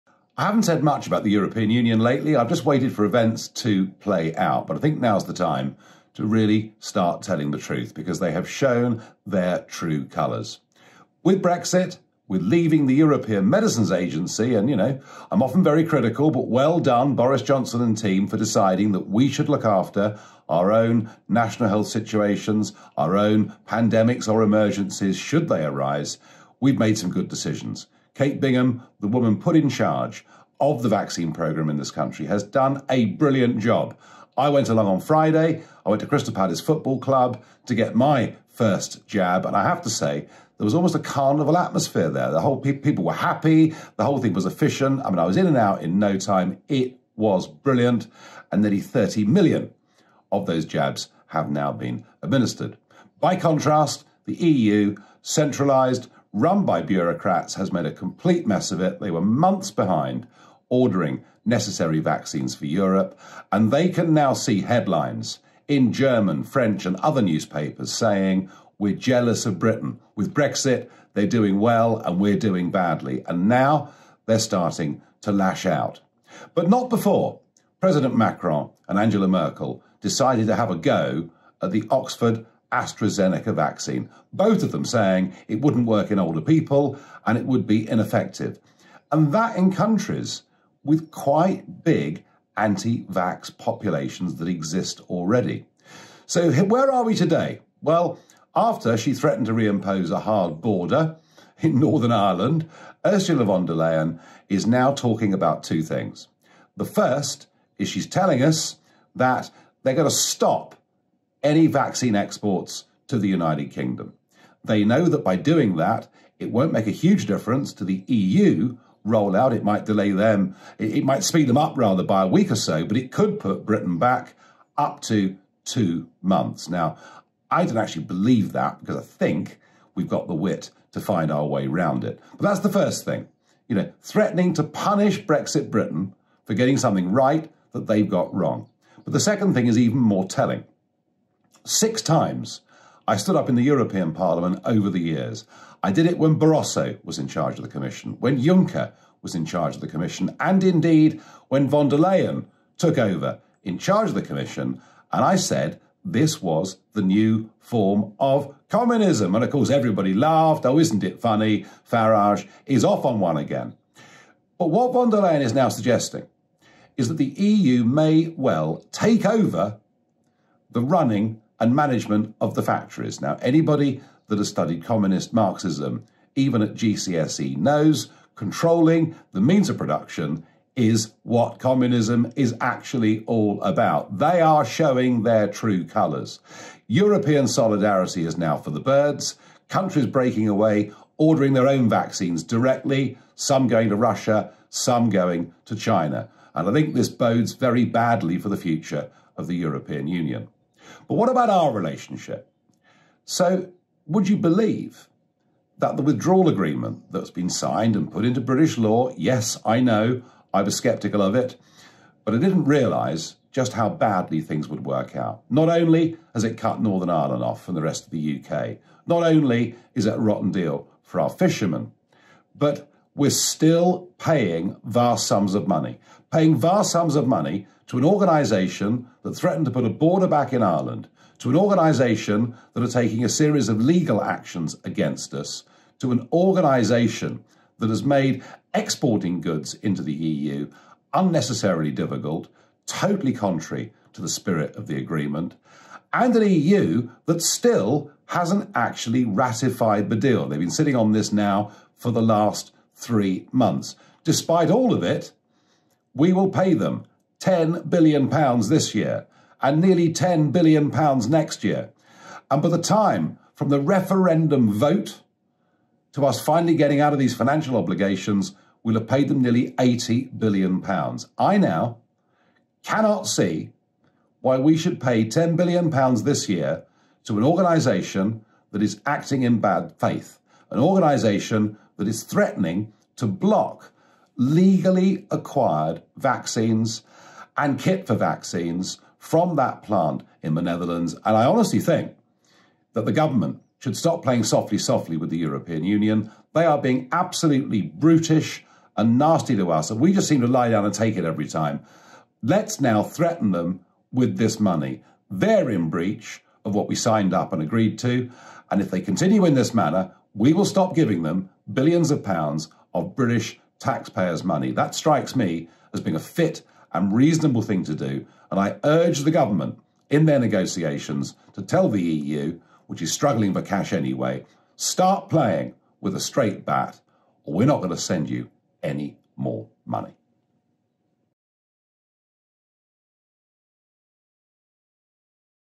Farage reacts as EU block exports.
The EU have shown their true colours once again. Thank goodness for Brexit.